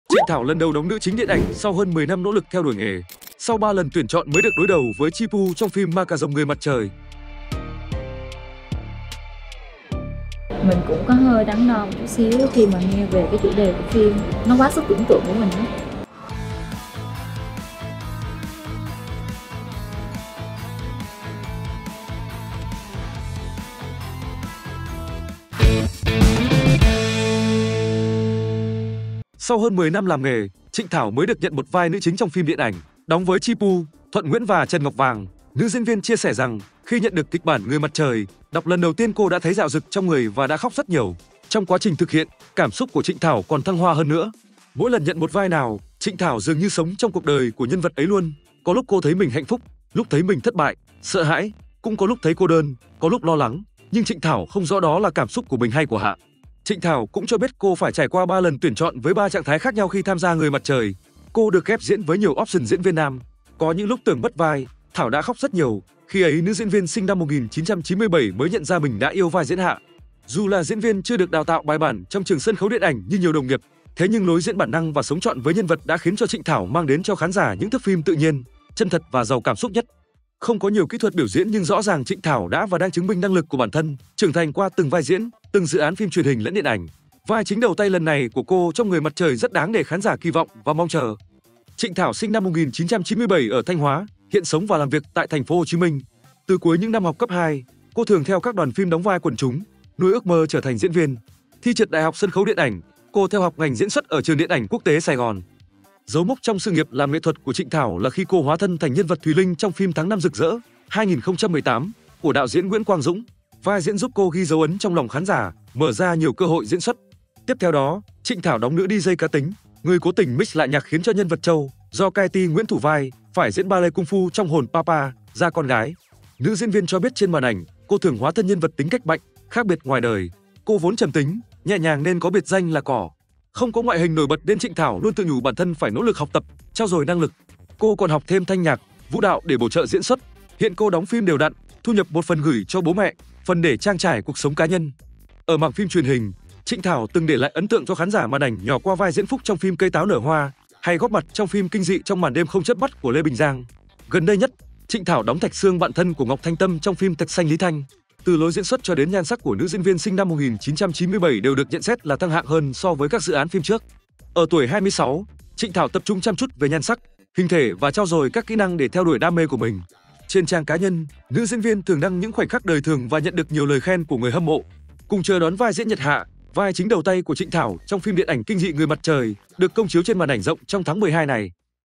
Trịnh Thảo lần đầu đóng nữ chính điện ảnh sau hơn 10 năm nỗ lực theo đuổi nghề
Sau hơn 10 năm làm nghề, Trịnh Thảo mới được nhận một vai nữ chính trong phim điện ảnh. Đóng với Chi Pu, Thuận Nguyễn và Trần Ngọc Vàng. Nữ diễn viên chia sẻ rằng, Khi nhận được kịch bản Người Mặt Trời, đọc lần đầu tiên cô đã thấy rạo rực trong người và đã khóc rất nhiều,. Trong quá trình thực hiện cảm xúc của Trịnh Thảo còn thăng hoa hơn nữa.

☞ Bản quyền thuộc về Vựa Muối Phim Việt.